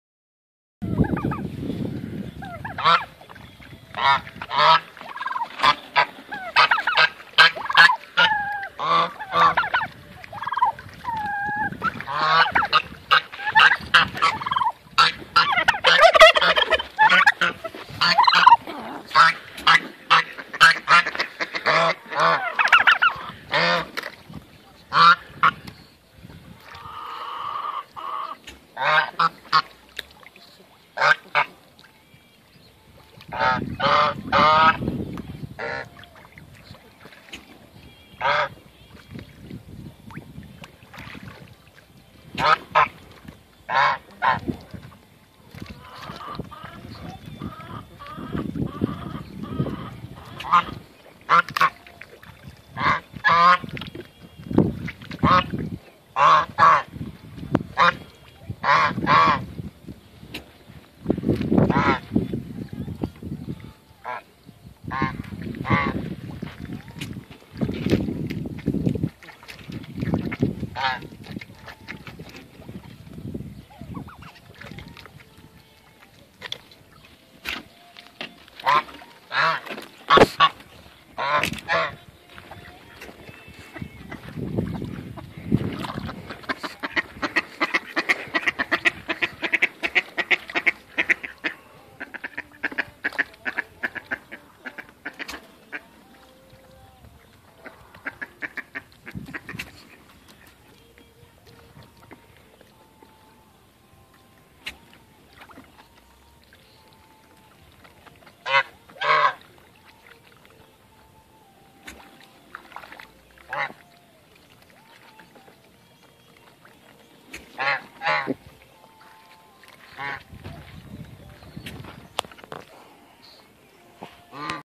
Clyde and his toy
Clyde playing in the pool with his toy